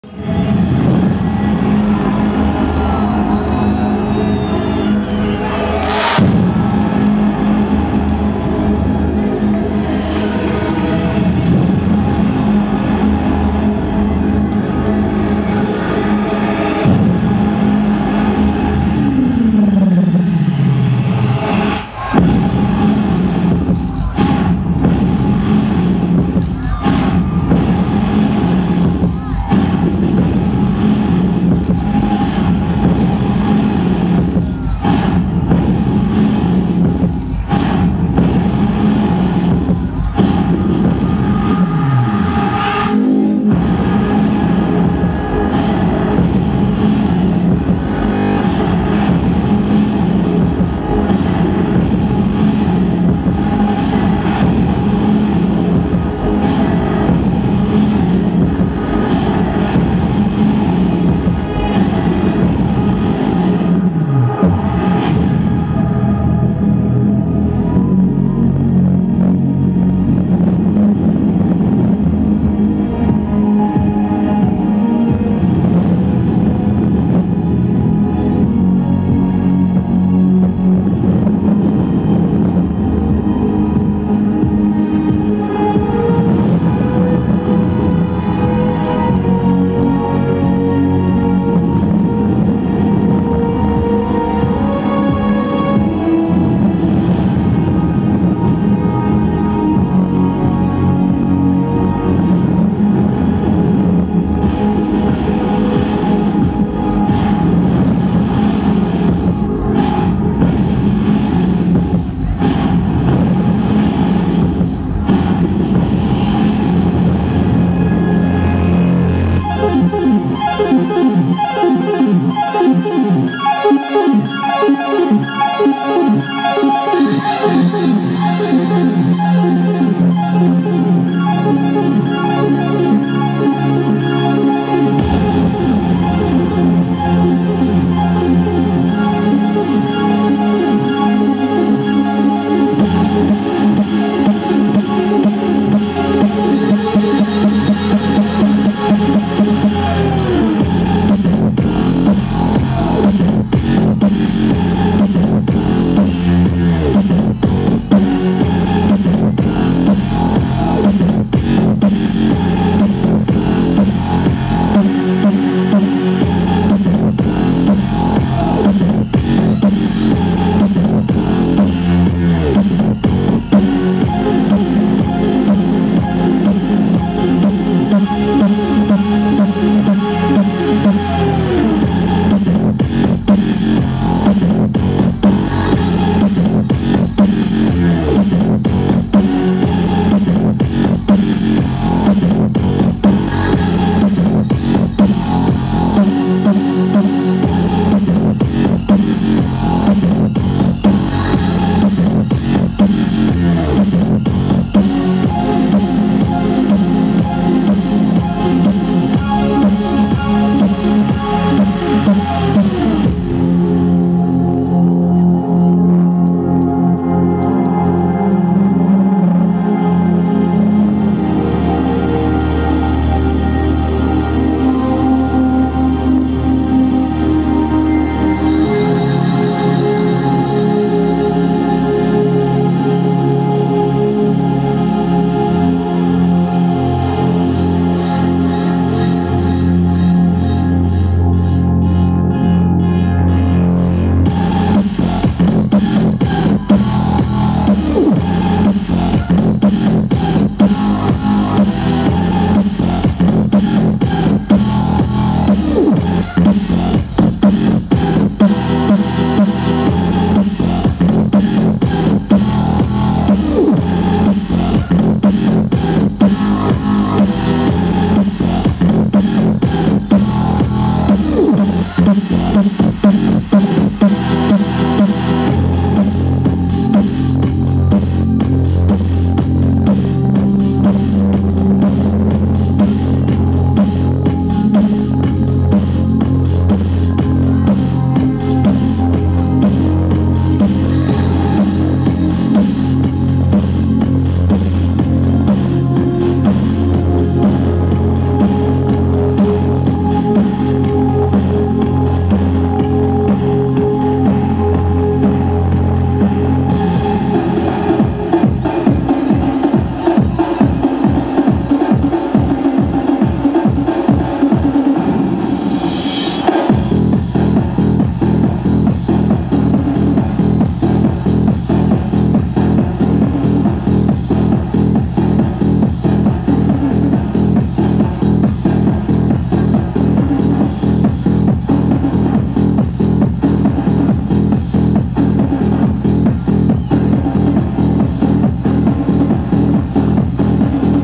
Лазерное шоу
Лазерное шоу в честь международного фестиваля народного творчества "Венок дружбы" на площади имени Ленина в Бобруйске